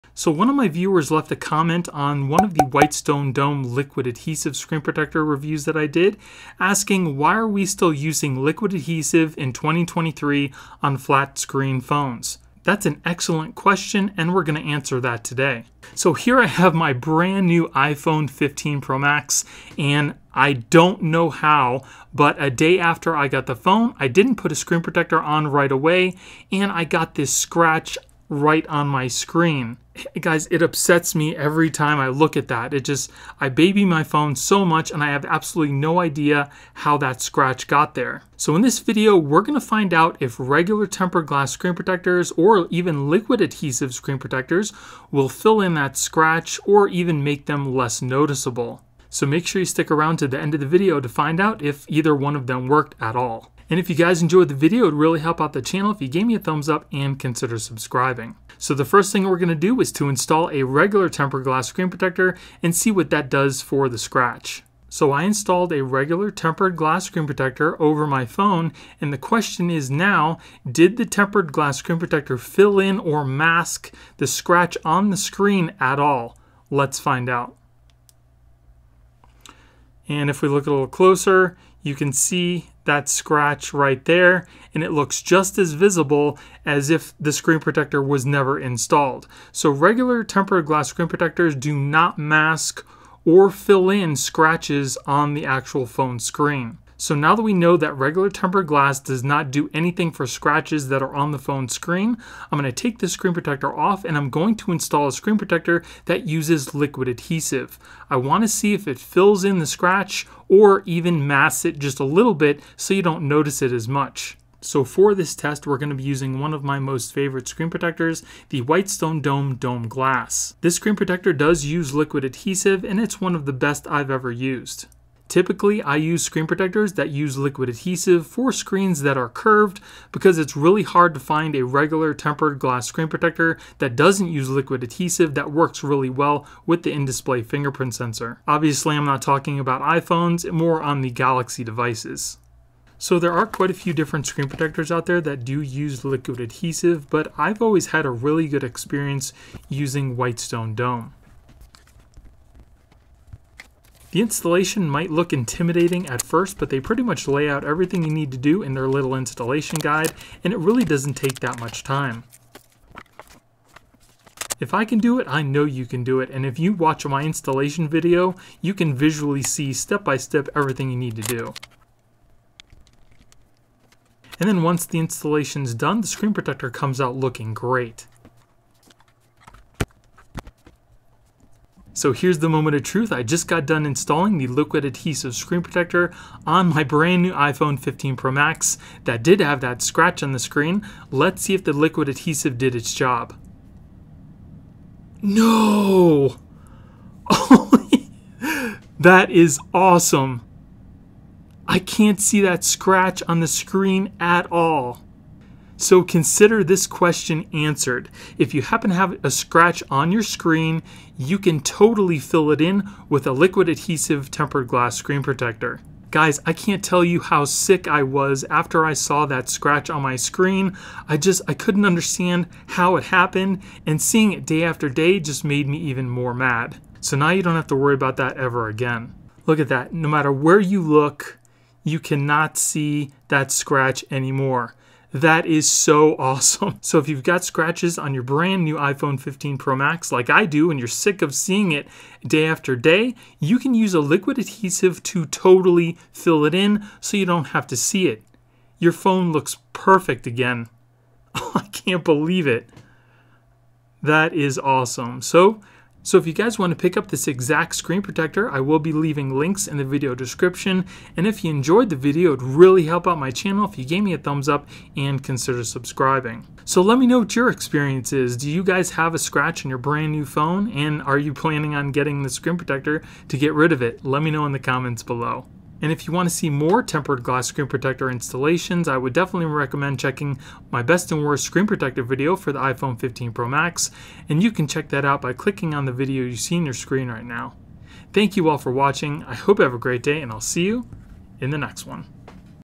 Liquid vs. Tempered Glass Protectors: Which Fixes Scratched Screens Better? - Ft. Whitestone Dome
I'm going to answer the question of why you would want to use a liquid adhesive screen protector over a tempered glass protector on a flat screen in 2023. It might be a cheap solution if you happen to have a scratched iphone 15 screen. Lets find out

Whitestone Premium Glass